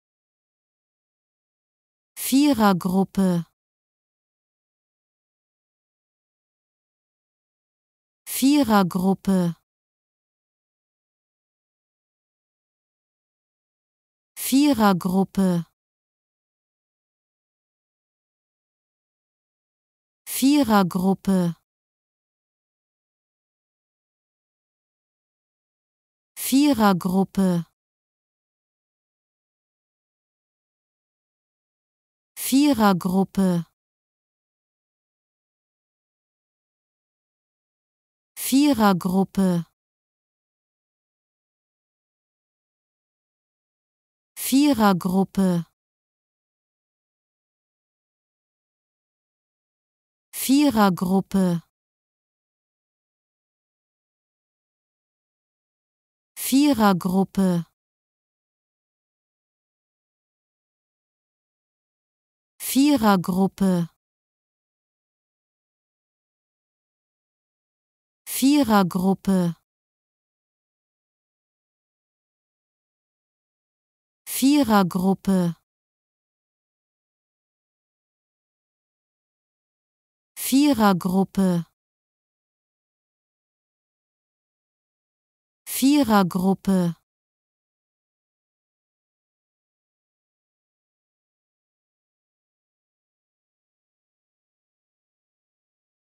How to Pronounce ''Vierergruppe'' (Group of four) Correctly in German
Learn how to say ''Vierergruppe'' in German with this short pronunciation tutorial.